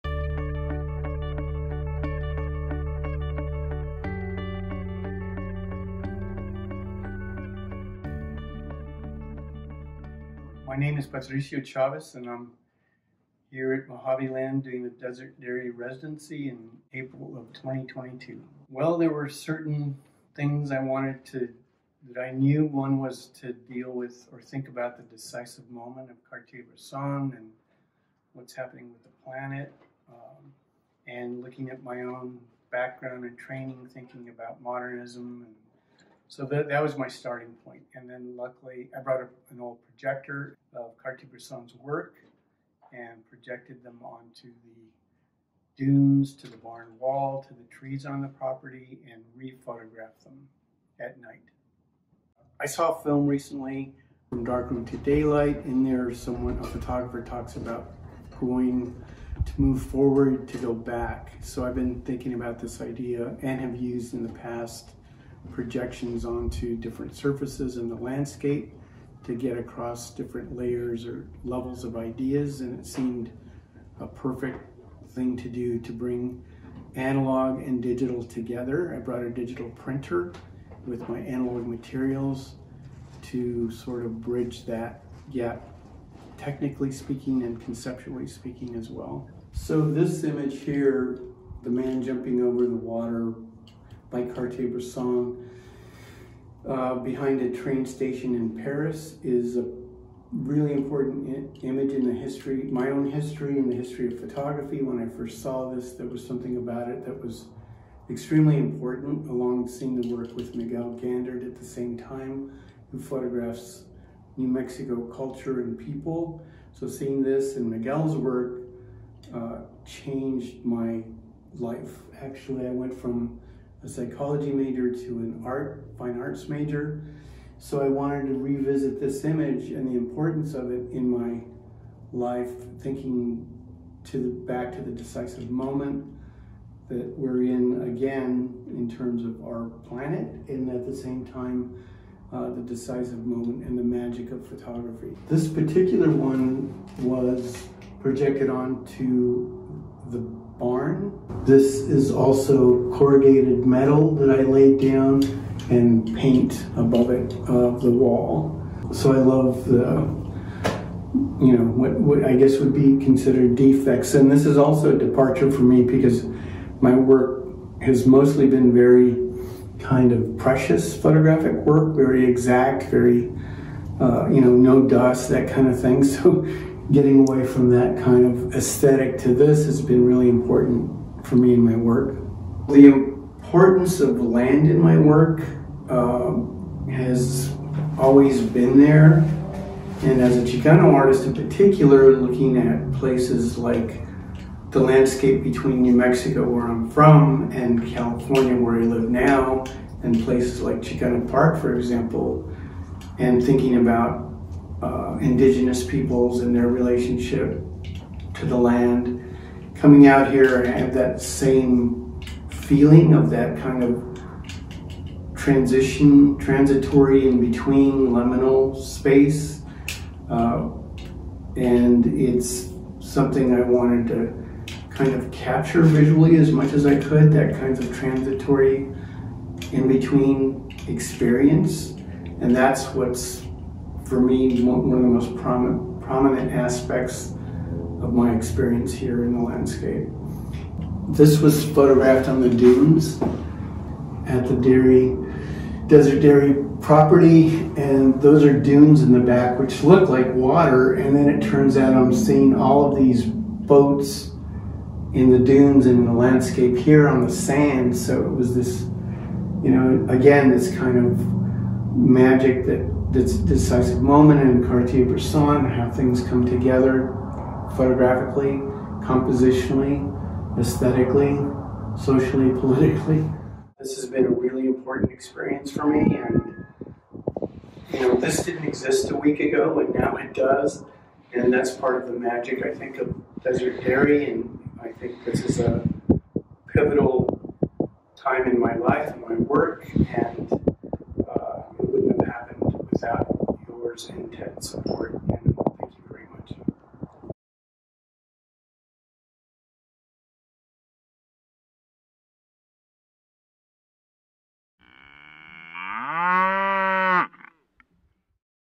Patricio Chávez at Desert Dairy Artist Residency
Desert Dairy Resident Artist Patricio Chávez discusses his new photographic prints and the process used to shoot them for his new piece, Henri Cartier Bresson -" The Decisive Moment".  At the end of his residency, he staged a popup exhibition of his work at the Mojaveland Gallery in 29 Palms.